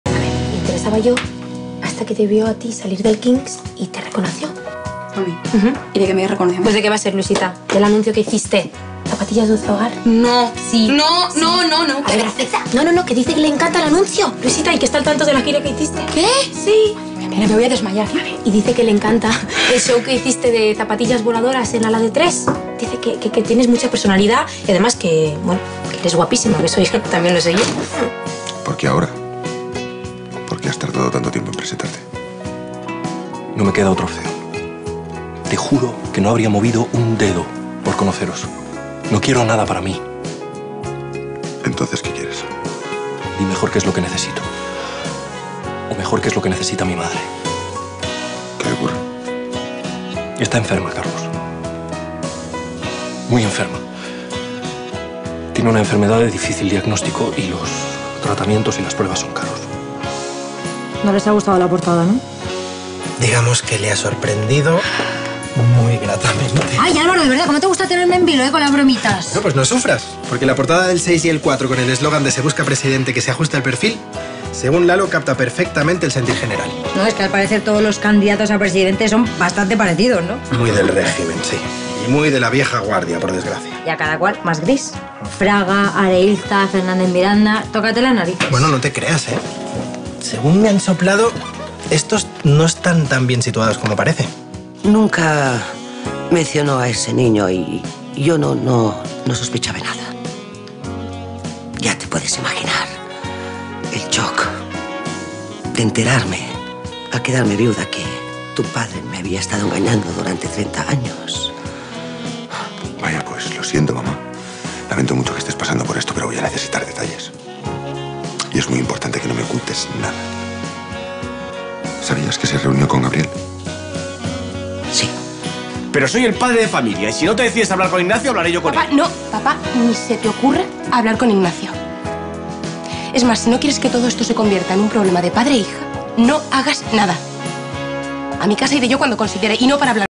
Vicente comienza a colaborar con Carlos y Quintero
Tras confesarle a Carlos que son hermanos, Vicente decide ayudar a Carlos y a Quintero con el caso de la muerte de Gabriel.